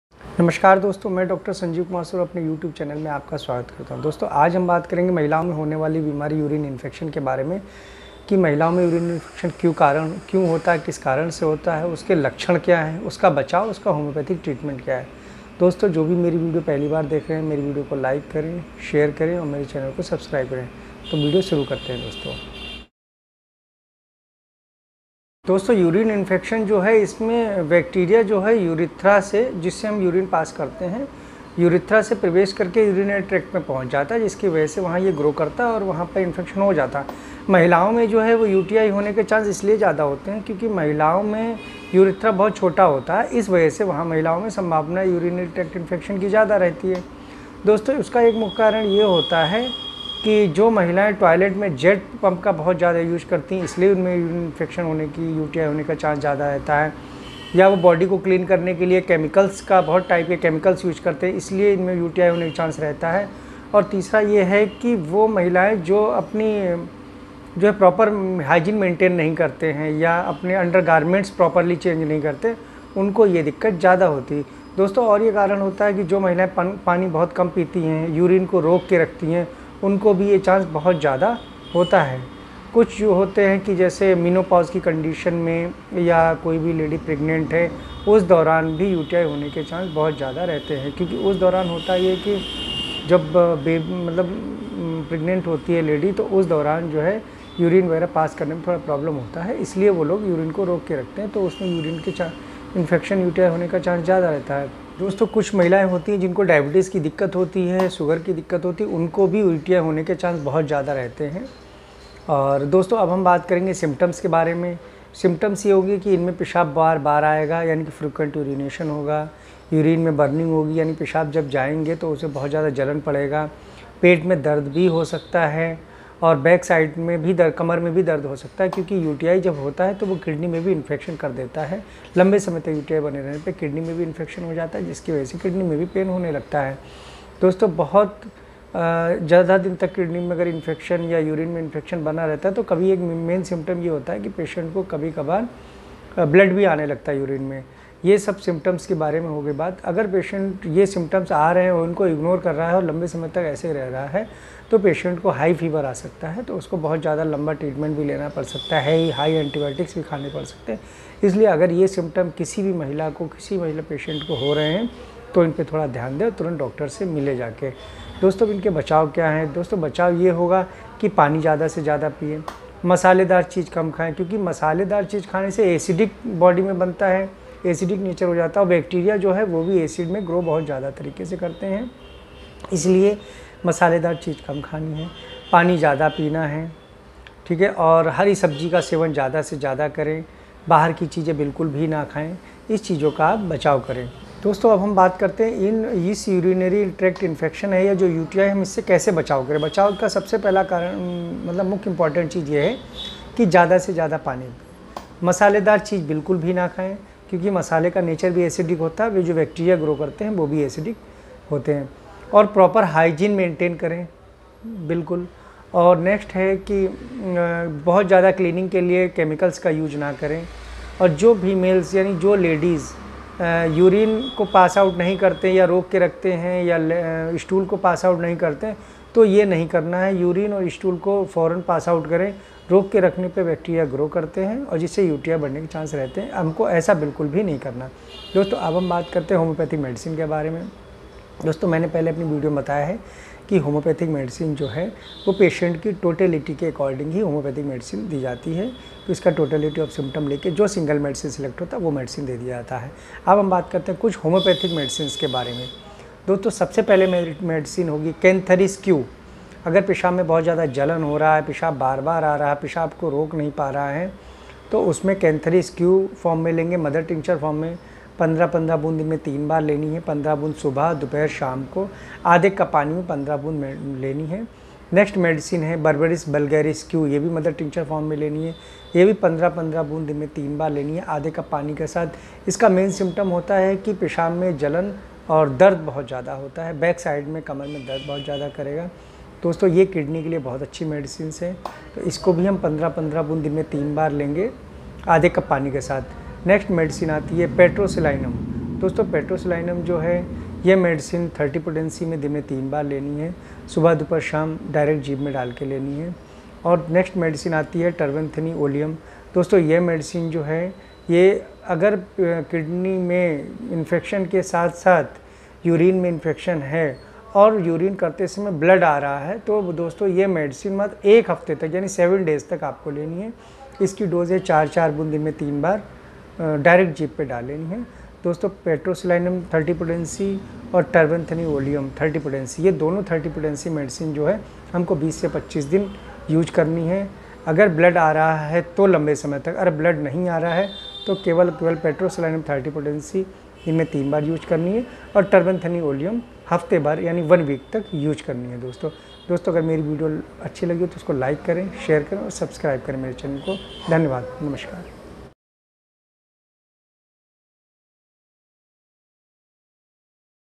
पेशाव नाली में इन्फेक्शन urinarytractinfections cystitis लक्षण कारण व होम्योपैथीक इलाज।
Hello friends , I am Dr. Sanjeev kumar Saroj.
watch this video know about U.T.I. Urinary tract infection symptoms and it's Homoeopathic treatment.
Homoeopathic medicine mentioned in the video and dosages

Important homoeopathic medicine...

Cantharis Q
Barberis vulgaris Q
Terebinthiane Oleum 30
Petroselinum 30

Note- This video is knowledge purpose only.

Please consult your doctors before taking the medicine.

Dr. Sanjeev Homoeopathy.